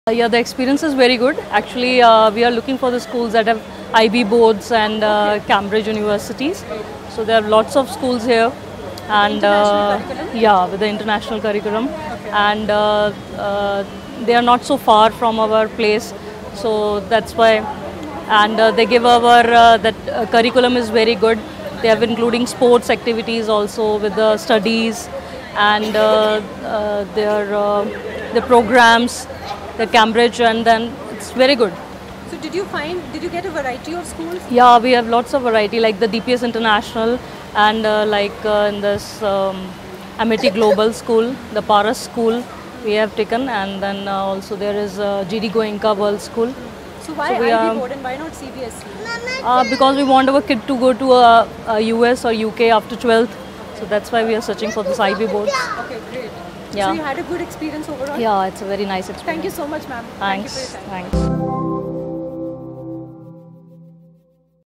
Premier Schools Exhibition - 2018
Visitor's experience during Educators & Parents Conclave at the Premier Schools Exhibition Apparel House, Sector - 44, Gurugram.